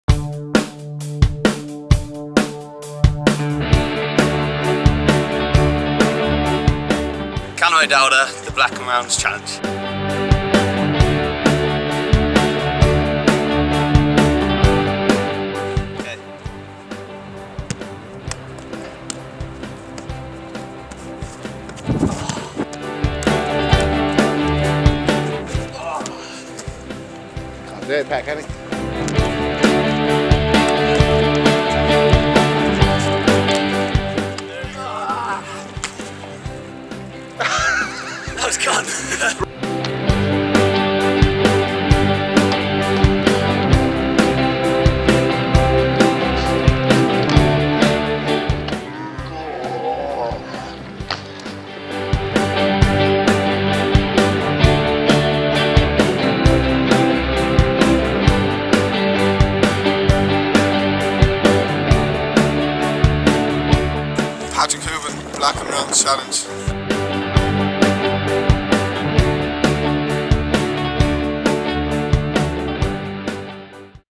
The Black n Rounds Challenge - Callum O'Dowda
Callum O'Dowda takes the Black n Rounds Challenge